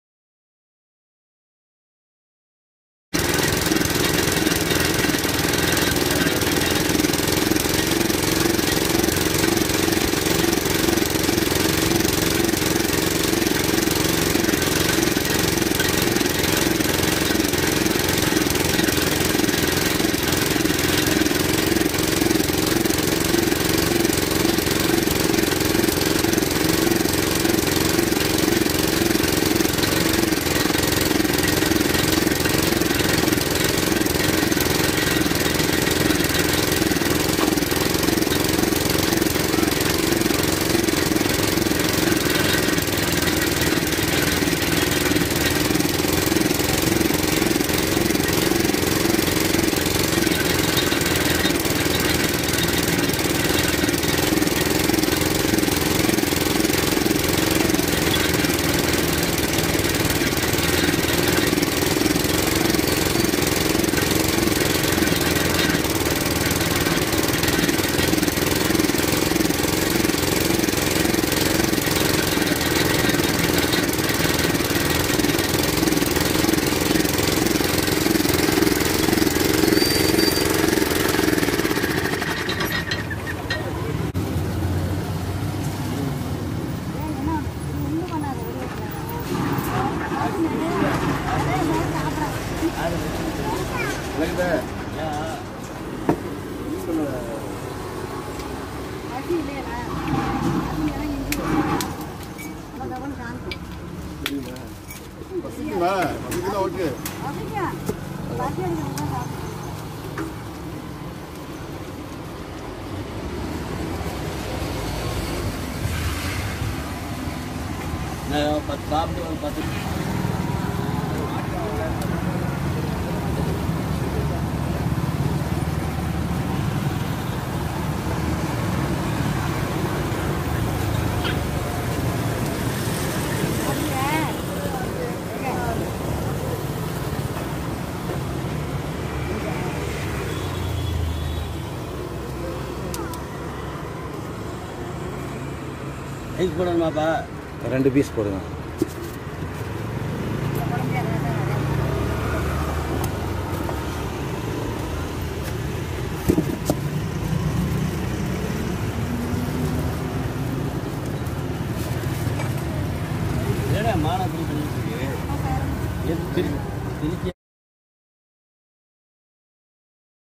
100% Fresh & Sweet! The Process Of Making Sugarcane Juice - 2022 India
100% Fresh & Sweet! The Process Of Making Sugarcane Juice  - 2022 India